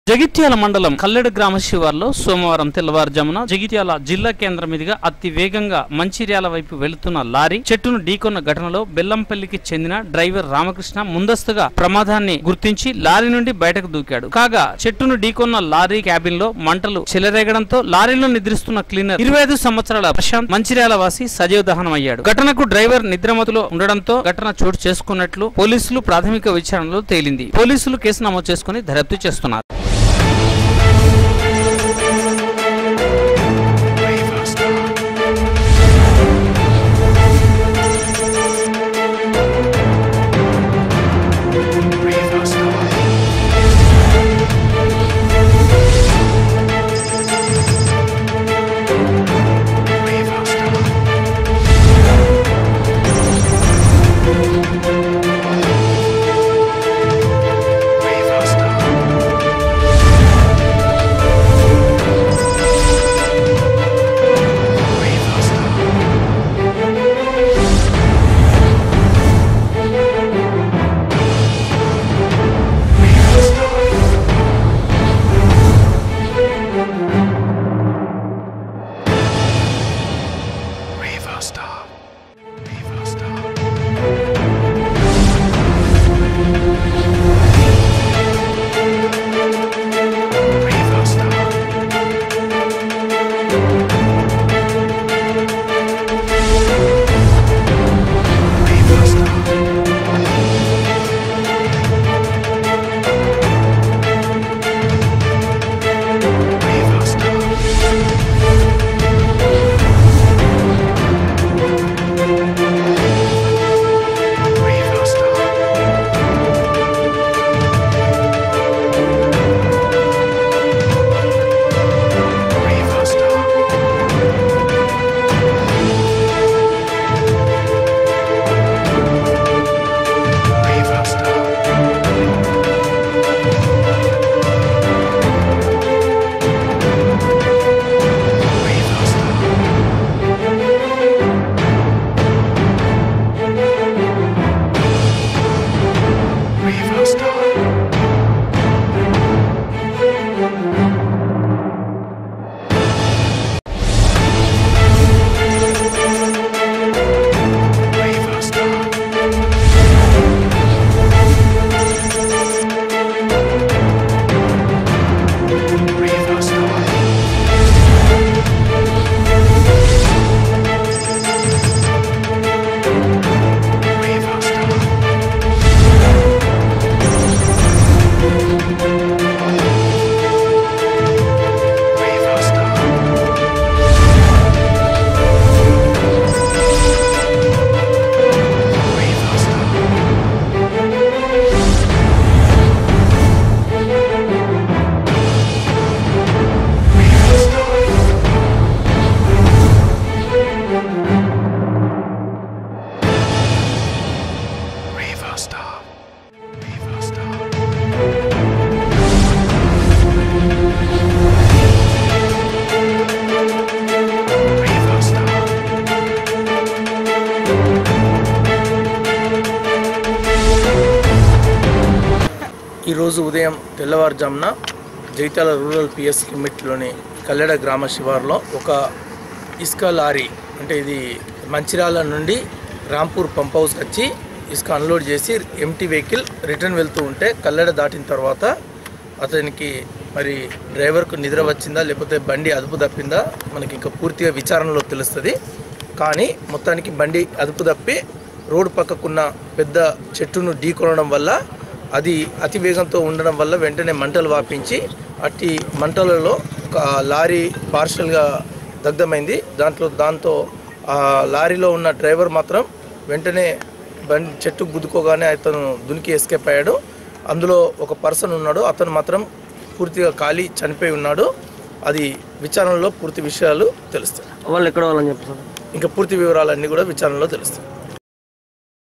06.05.19| DSC | Jagtial |జగిత్యాల మండలం కల్లెడ గ్రామం వద్ద చెట్టును ఢీకొన్న లారీ క్లీనర్ సజీవదహనం
06.05.19| Durga Siti Cable | News |జగిత్యాల మండలం కల్లెడ గ్రామం వద్ద చెట్టును ఢీకొన్న లారీ క్లీనర్ సజీవదహనం